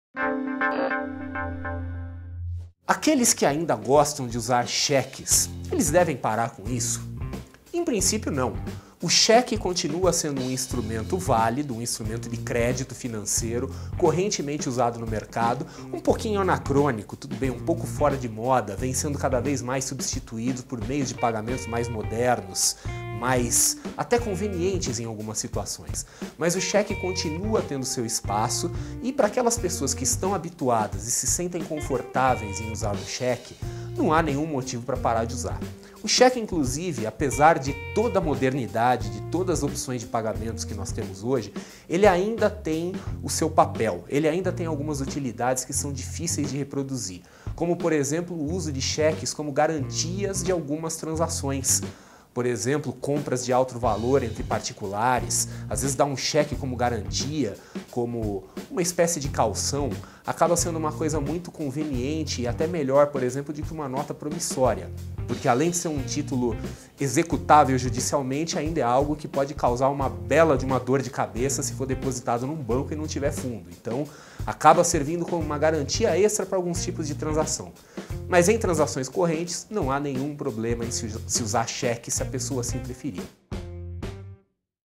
Eu ainda gosto de usar cheque. Devo parar?
São Paulo – Apesar do forte estímulo ao cartão de crédito, você ainda gosta de usar o bom e velho cheque? Veja se vale a pena no vídeo de finanças pessoais de hoje, com o consultor financeiro André Massaro.